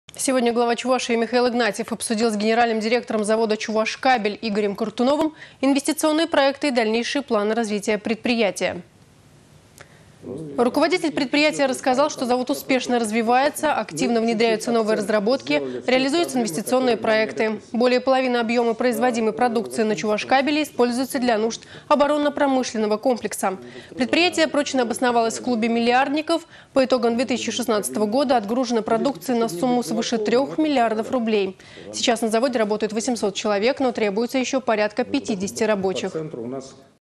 Рабочая встреча Главы Чувашии с генеральным директором завода «Чувашкабель»
Сегодня Глава Чувашии Михаил Игнатьев обсудил с генеральным директором завода «Чувашкабель» Игорем Кортуновым инвестиционные проекты и дальнейшие планы развития предприятия.